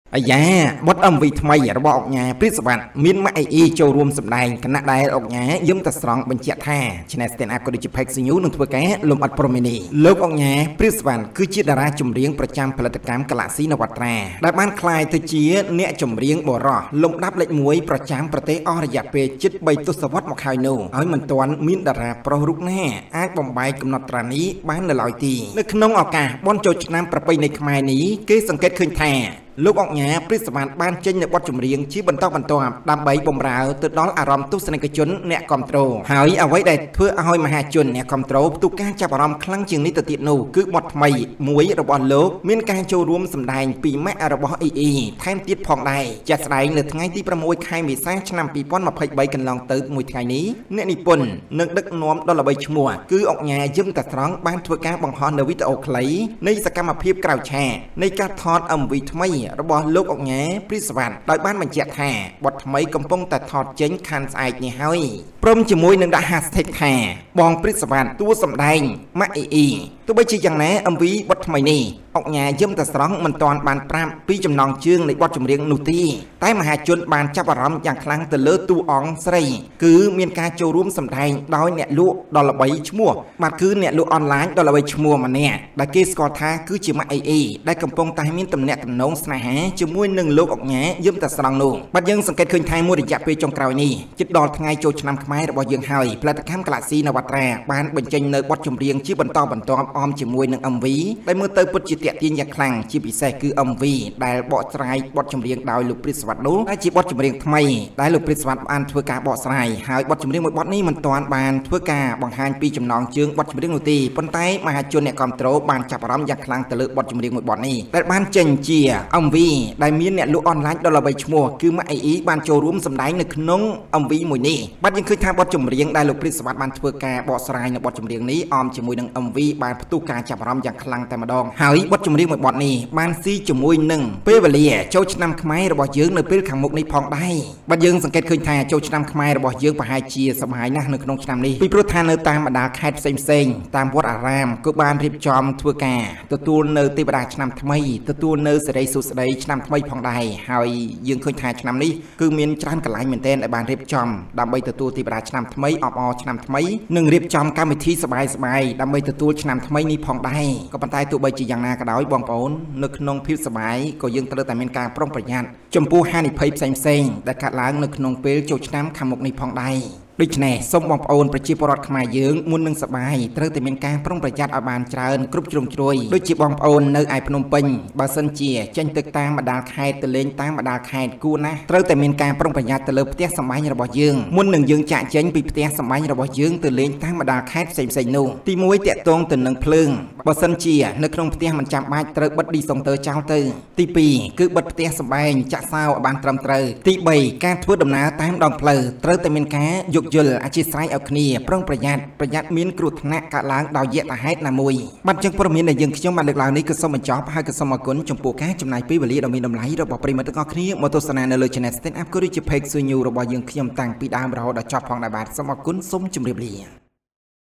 ​ក្តៅៗ MVថ្មី​ឧកញ៉ា​ ព្រាប សុវត្ថិ មាន​ម៉ាក់អ៊ីអ៊ី ចូលរួមសម្ដែង ខណៈ​ឧកញ៉ា ​យឹម តាស្រង់ បញ្ជាក់ថា…,
​ក្តៅៗ MVថ្មី​ឧកញ៉ា​ ព្រាប សុវត្ថិ មាន​ម៉ាក់អ៊ីអ៊ី ចូលរួមសម្ដែង ខណៈ​ឧកញ៉ា ​យឹម តាស្រង់ បញ្ជាក់ថា…, Stand Up